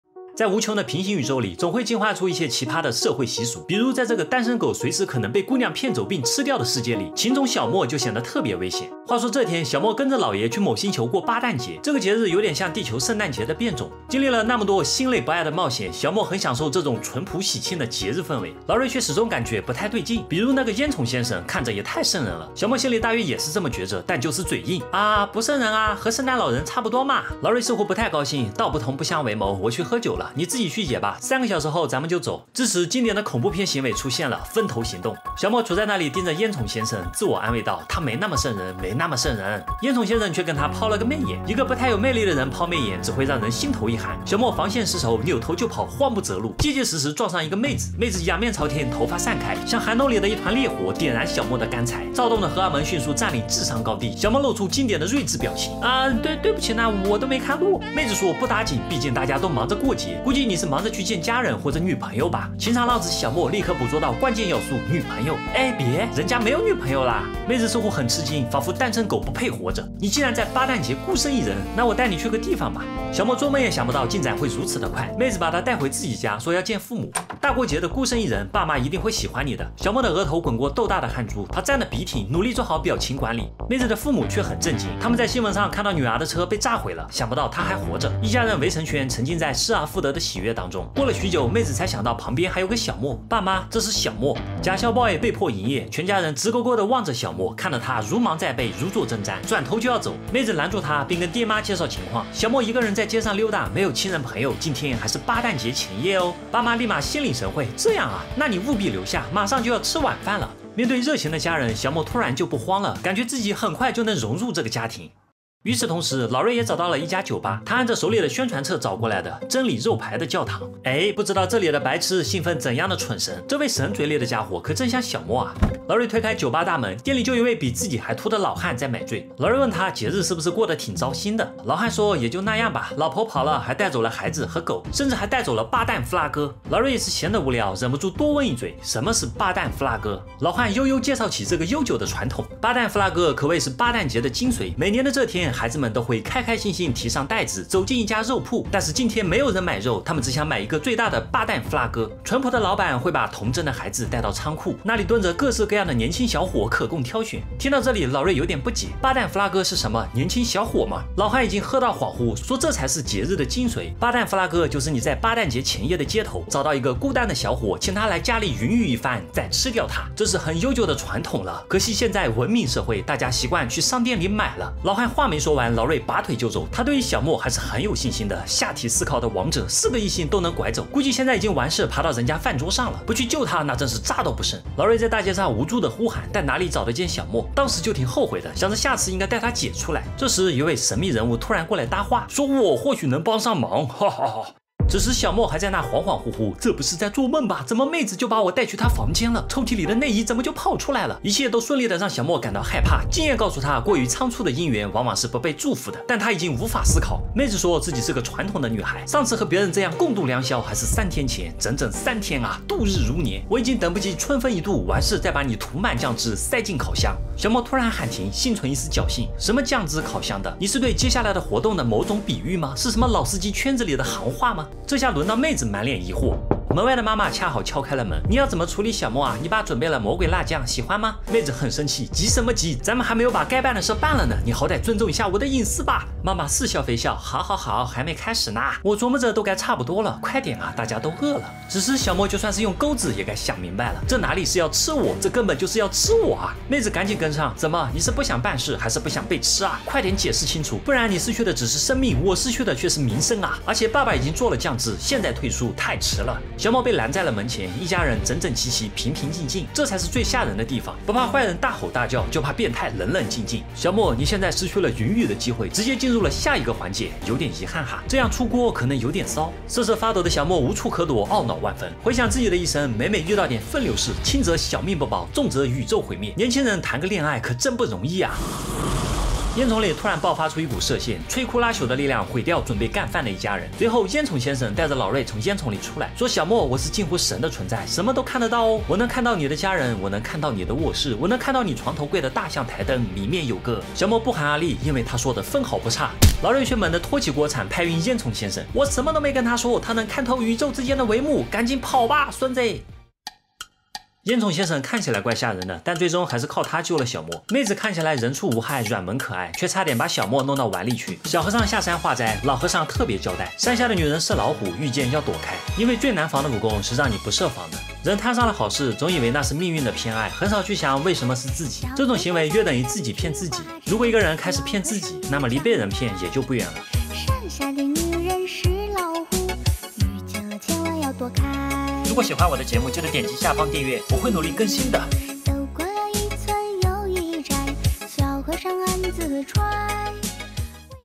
【扒】一次送命的邂逅，小莫的噩梦桃花运！瑞克和莫蒂漫画之致命邂逅
《瑞克和莫蒂》漫画故事5：小莫在外星邂逅妹子，不曾想却是一次致命的邂逅，这个星球的单身汉不配活着，噩梦桃花运让小莫很受伤。
漫画故事4 资本家瑞克之死：https//youtu.be/bN8B8mWoui0
🔗相关合集链接：